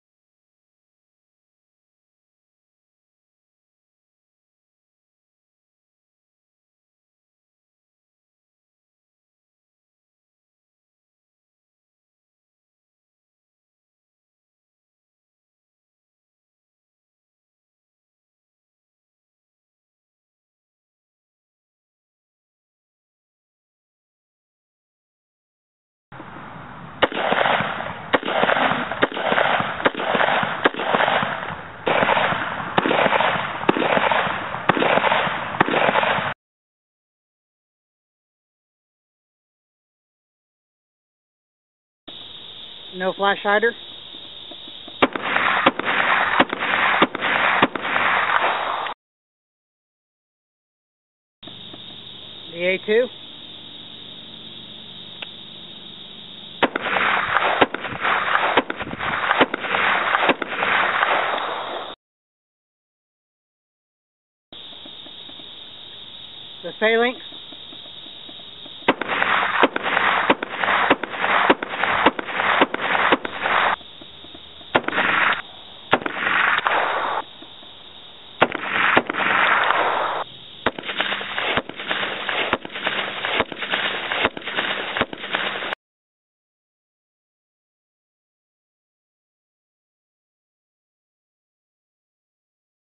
Addax Tactical Phalanx Flash Hider
This is a short video of the new Phalanx Flash Hider from Addax Tactical.